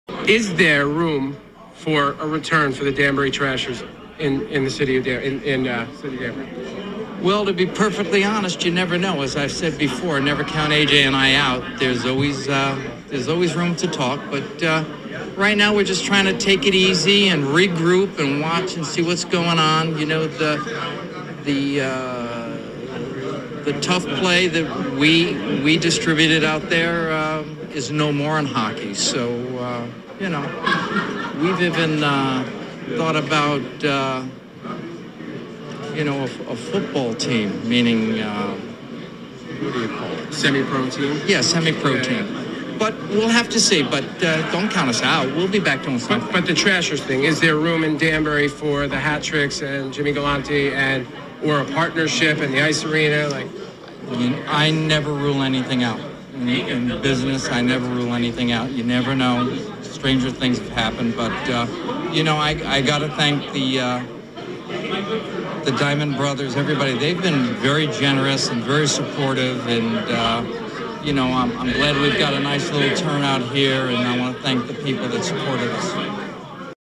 Could the Trashers Return to Danbury? Jimmy Galante: 'Never Count AJ and I Out'
Could the Trashers hockey team ever back a come back in Danbury?W We right to the source.